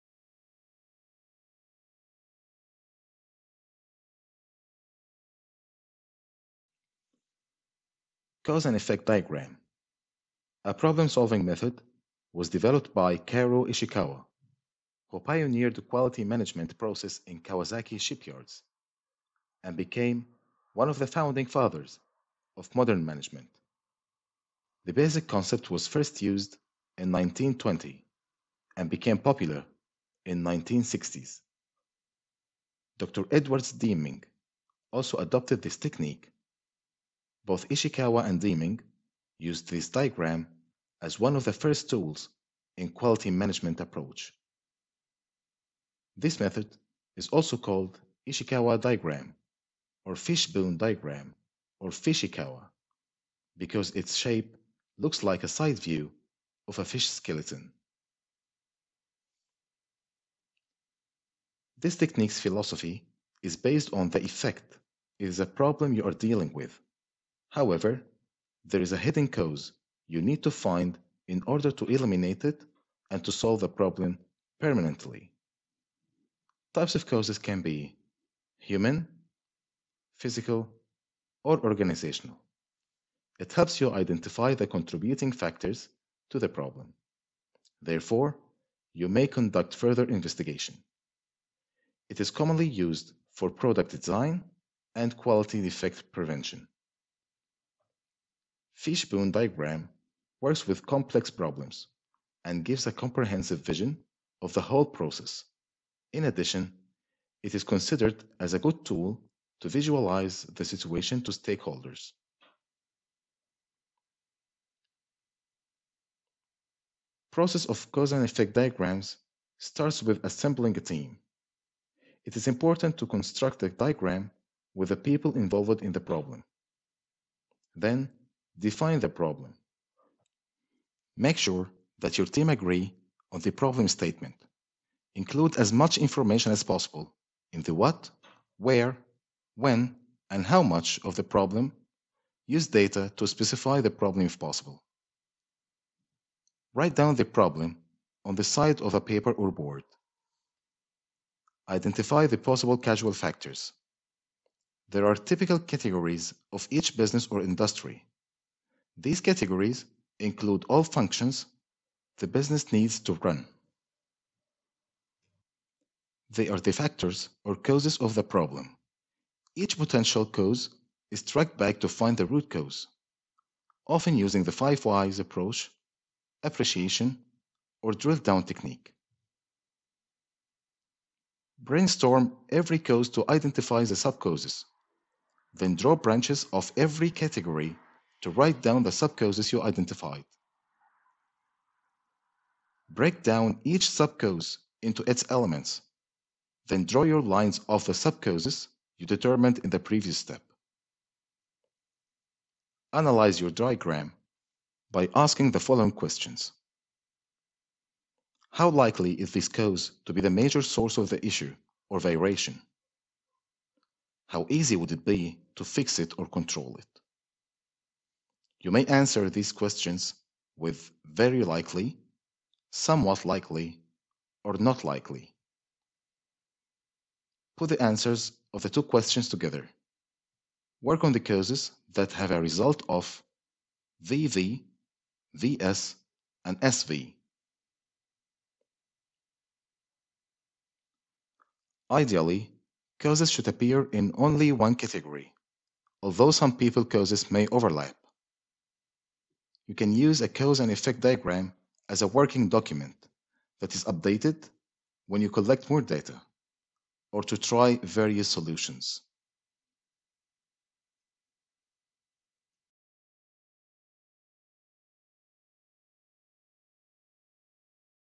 Fish bone diagram (cause and effect)
Cause and effect diagram is a perfect tool to analyze complex problems. It was developed by the pioneer "Karou Ishikawa" and adopted by Dr. Edwards Deming. it became popular in 1960s and still used in the largest organizations.